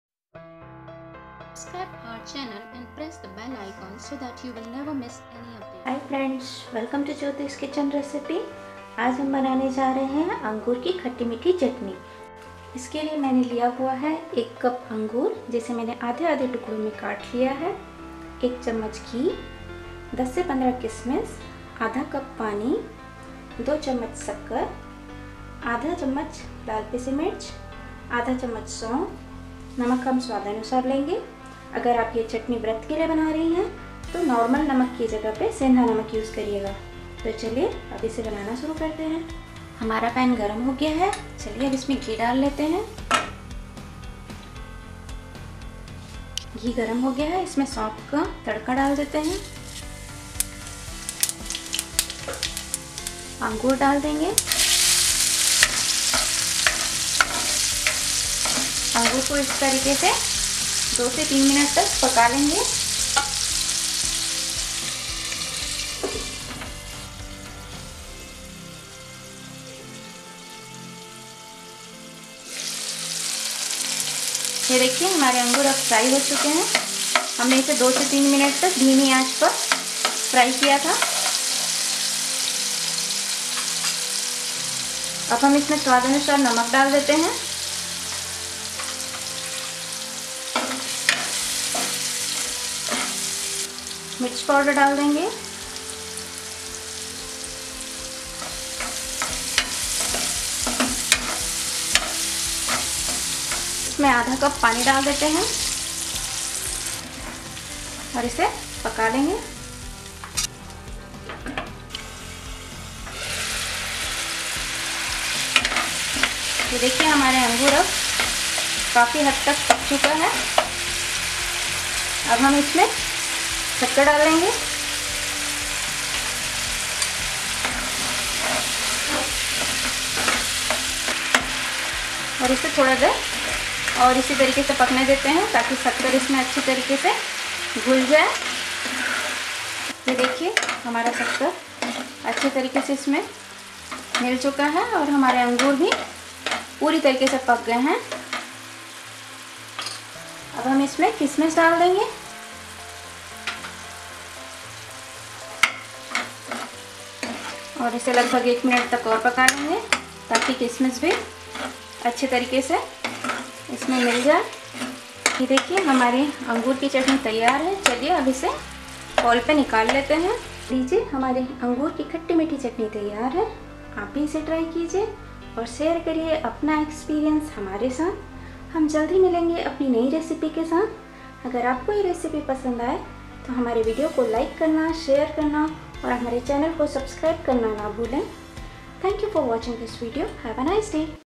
Angoor Ki Chutney |Grapes Chutney|अंगूर की खट्टी-मीठी स्पाइसी चटनी | Spicy Grapes Chutney
Today we will learn about how to prepare Angoor Ki Chutney |Grapes Chutney|अंगूर की खट्टी-मीठी स्पाइसी चटनी |Mouth Watering Spicy Grapes Chutney|